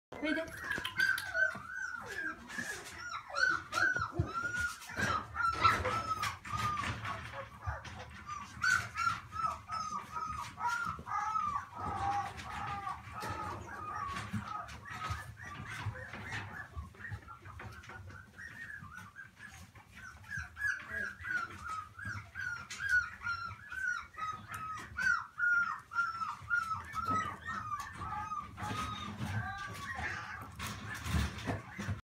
シェルティー仔犬
ひなの仔犬
おもちゃ大好き
明るい仔犬